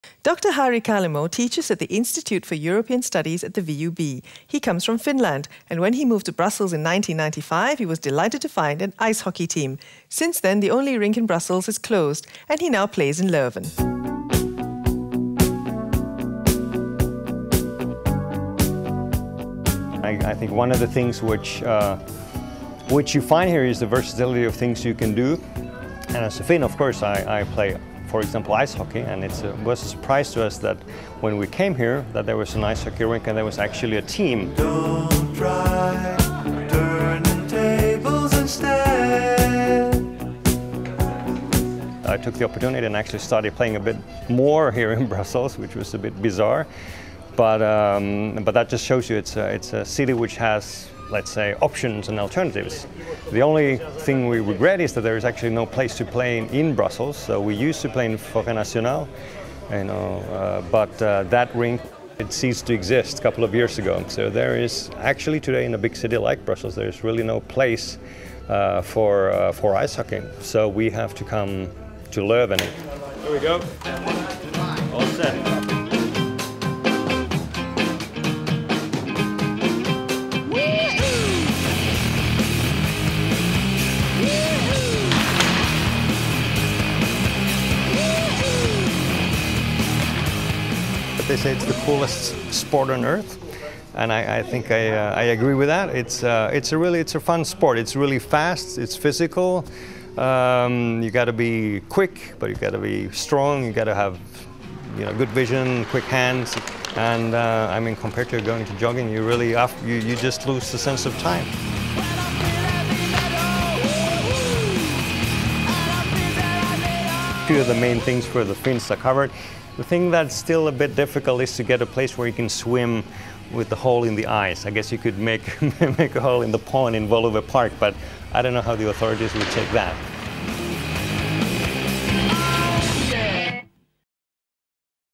MyBrussels Harri Kalimo
Dr. Harri Kalimo teaches at the Institute for European Studies at the VUB. He comes from Finland, and when he moved to Brussels in 1995 he was delighted to find an ice hockey team. Since then, the only ice hockey rink in Brussels closed and he now plays in Leuven.